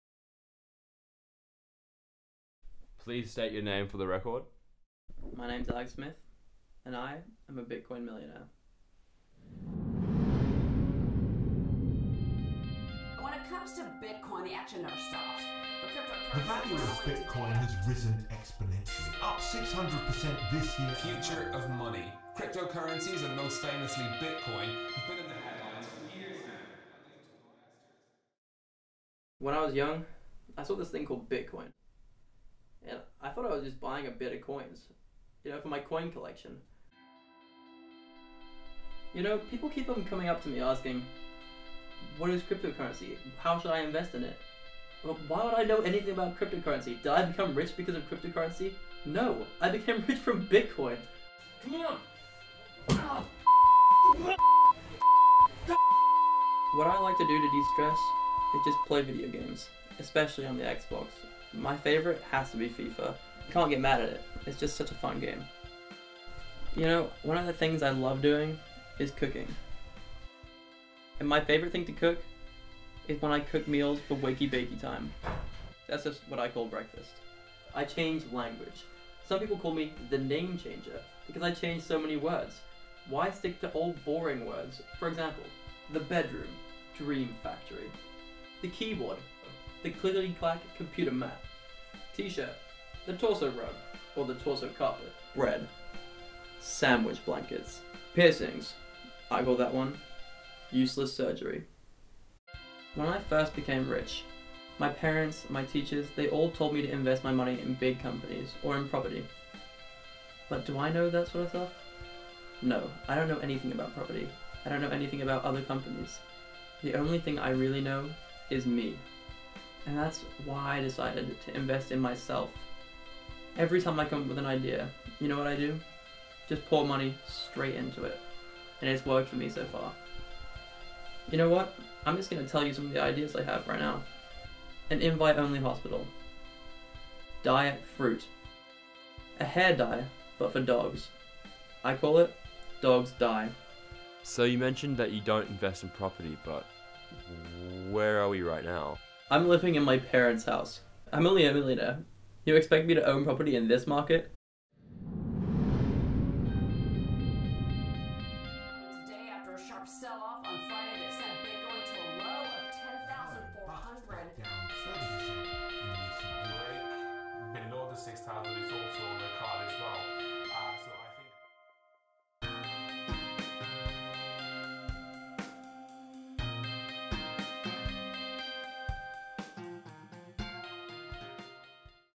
Bitcoin Boy
A short mockumentary for Oxford Brooke's Digital Media Production  course